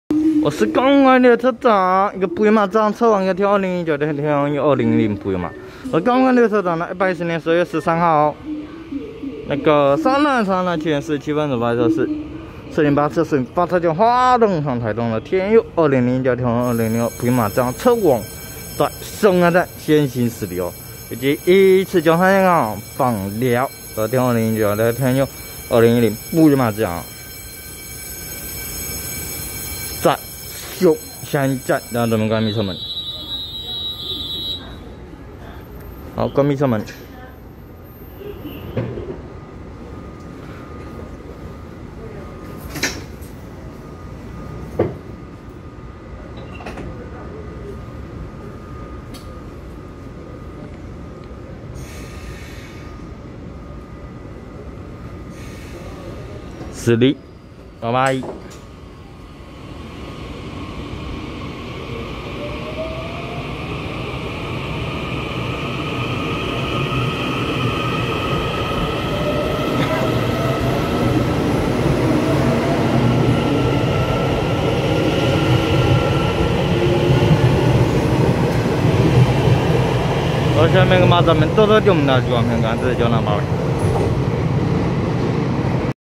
408次樹林發車經由花東線開往台東的TEMU2001車王+TEMU2002普悠瑪自強號，和111次南港發車經由山線開往枋寮的TEMU2009+TEMU2010普悠瑪自強號
110/12/13松山站7:47分拍攝408次樹林發車經由花東線開往台東的TEMU2001車王+TEMU2002普悠瑪自強號，和111次南港發車經由山線開往枋寮的TEMU2009+TEMU2010普悠瑪自強號